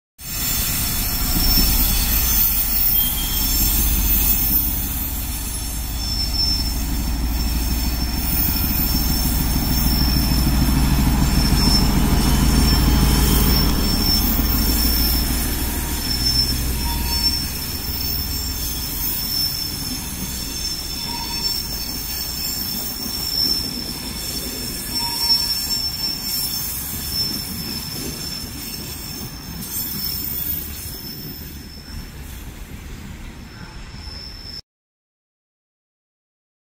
Rozjezd 754.067 - Brno hl. n.
Rozjezd Brejlovce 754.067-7 na Sp 1733 ze stanice Brno hl. n.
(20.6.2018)